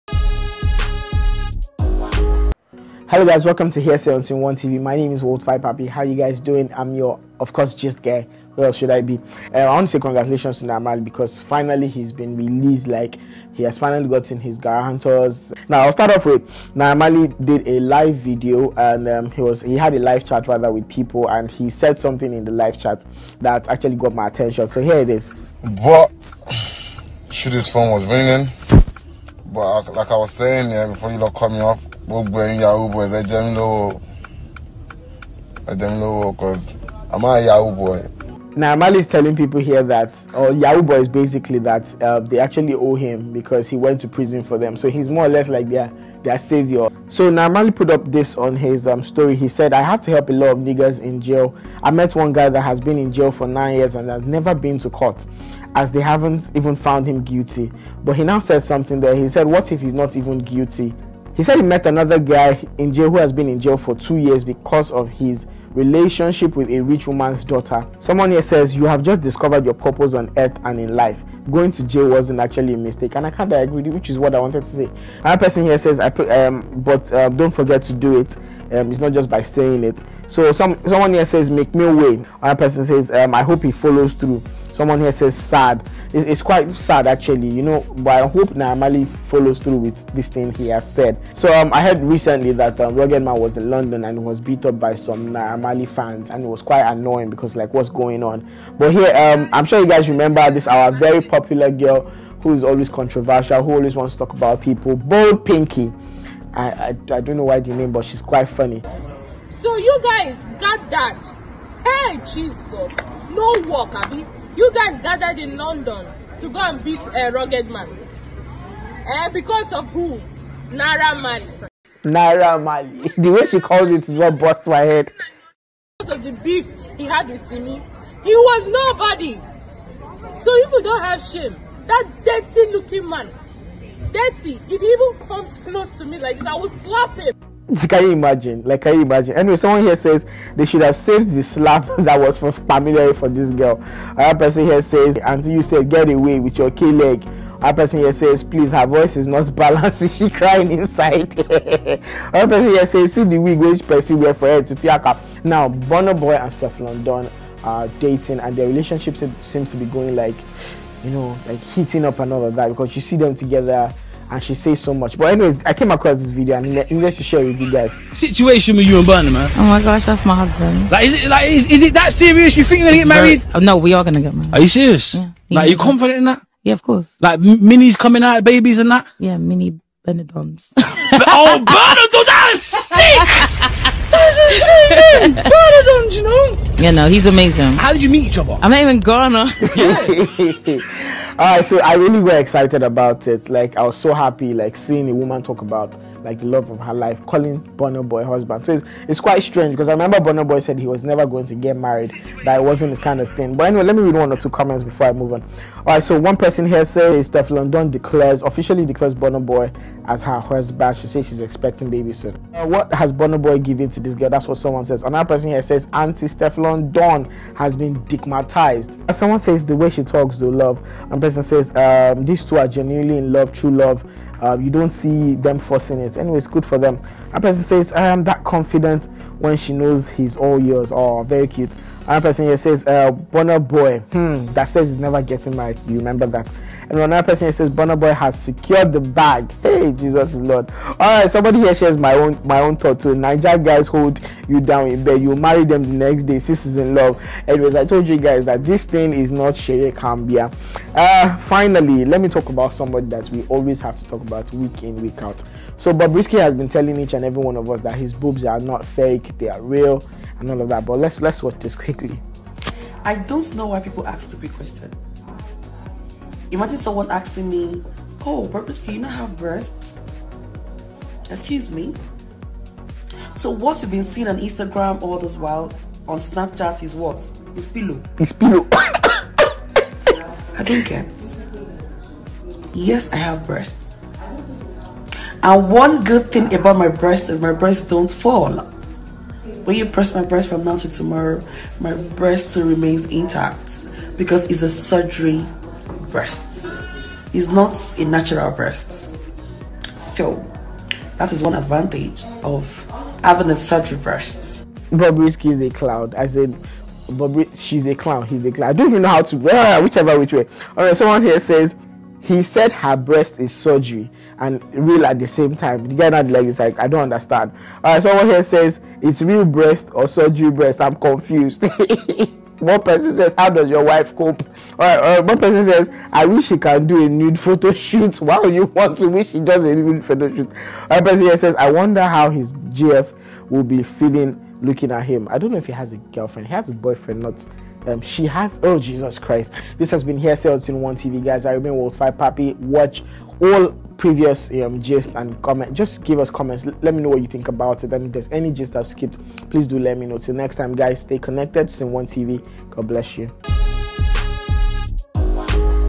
Naira Marley Regains Freedom, RuggedMan assaulted by 4 men, Bobrisky confirms Breasts are real
Naira Marley says Yahoo boys owe him money,
A girl has this to say about the fans that beat up Rugged Man in London.
What Naira Marley's Fans Did To Rugged Man,
BurnaBoy & girl friend, Steflon Don are expecting a baby,
Is Bobrisky's Breast Real or Fake ?